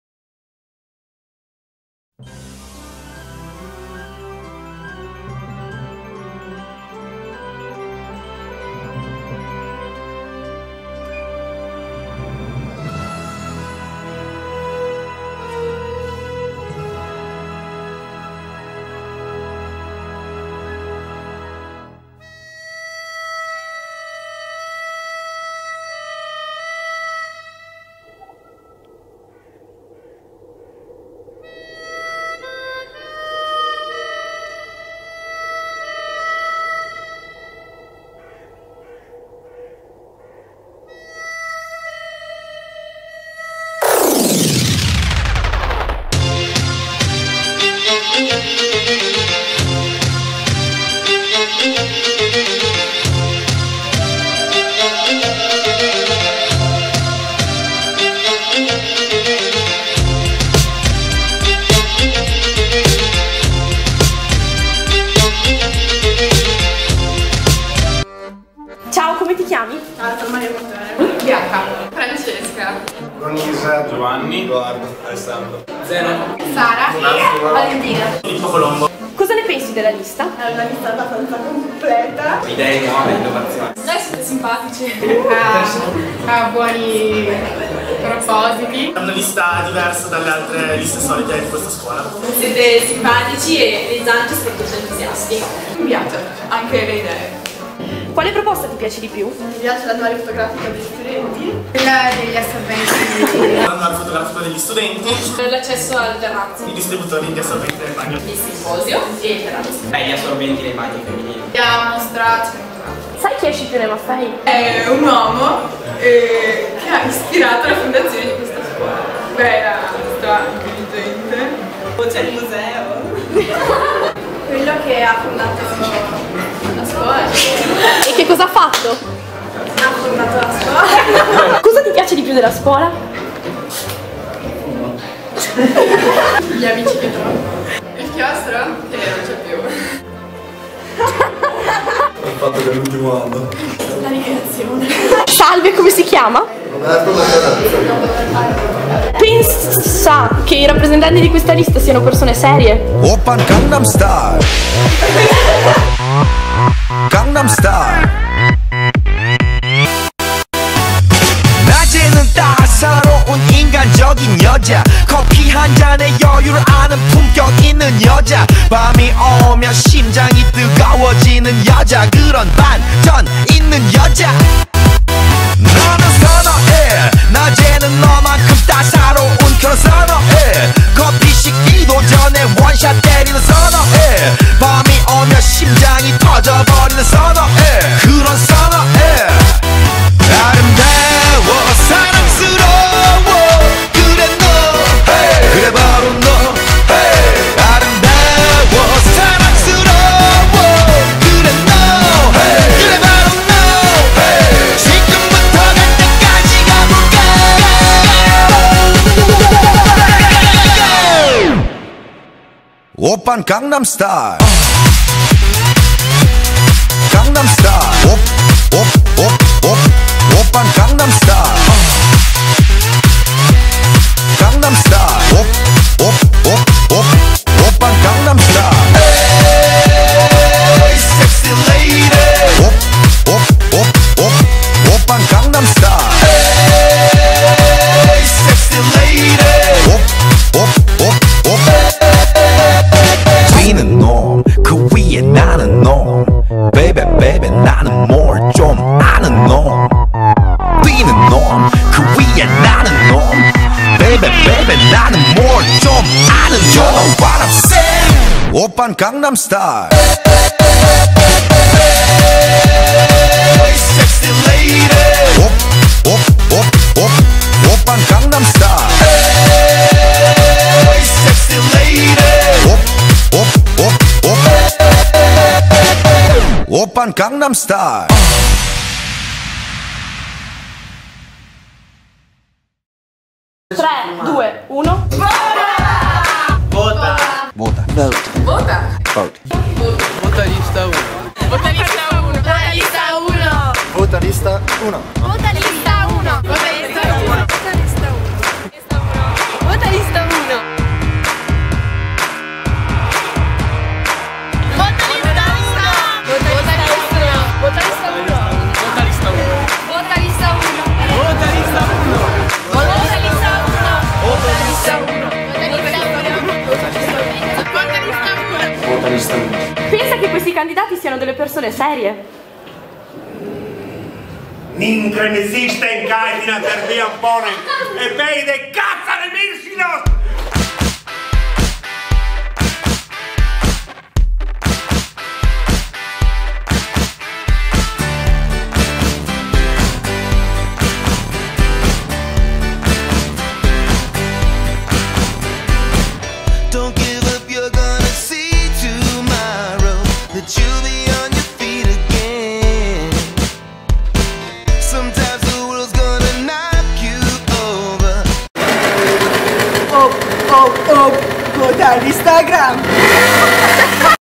LISTAGRAM (Liceo Maffei 2012/2013)
Liceo Scipione Maffei 20012/13
Video per l'elezione dei rappresentanti d'istituto.